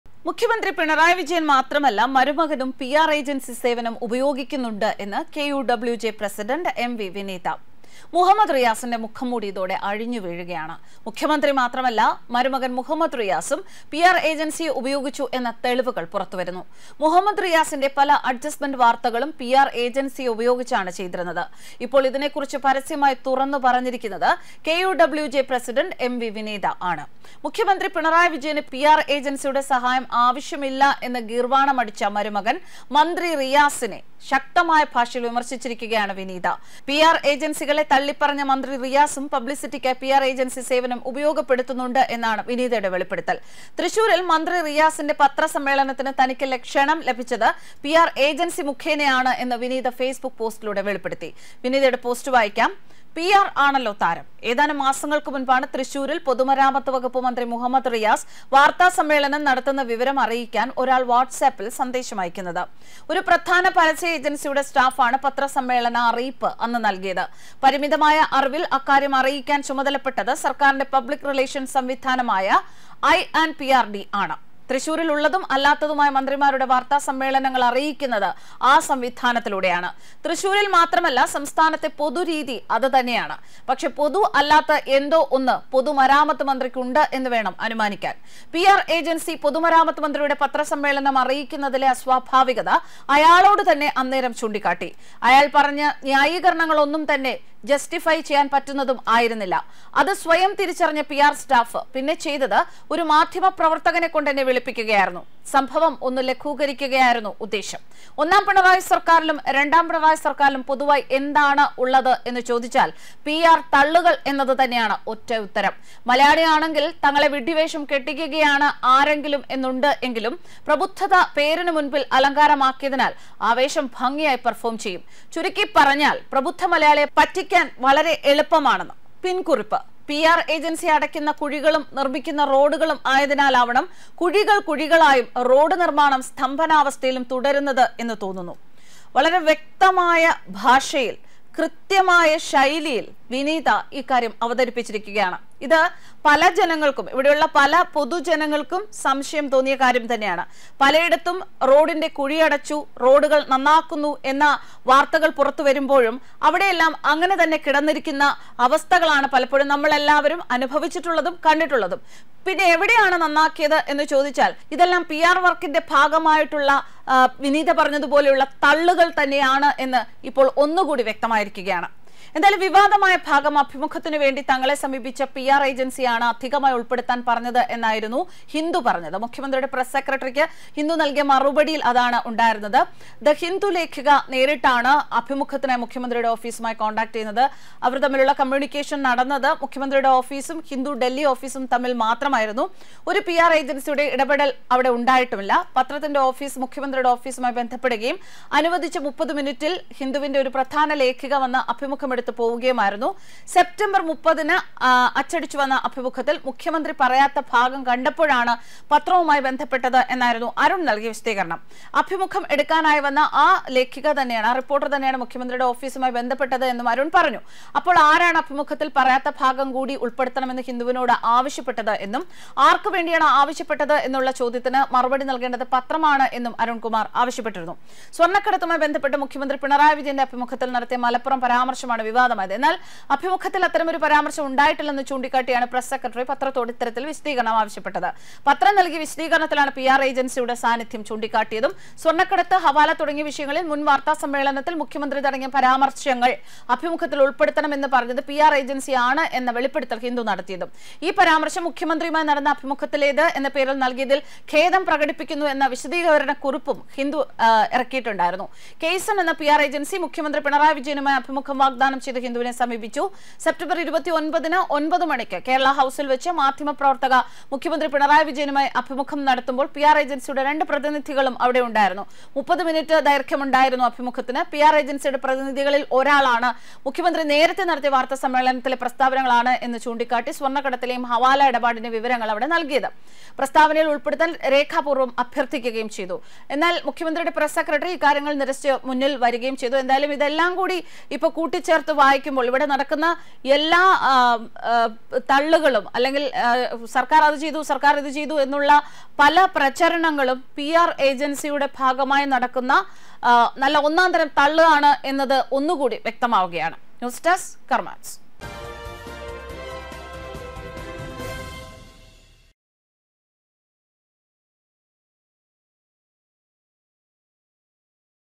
PR ഇല്ലായിരുന്നെങ്കിൽ മരുമോന്റെ റോഡിലെ കുഴികൾ കുഴികളായ് കിടന്നേനെ
ഒന്നാം പിണറായി സർക്കാരിലും രണ്ടാം പിണറായി സർക്കാരിലും പൊതുവായി ഉള്ളത് PR തള്ളുകൾ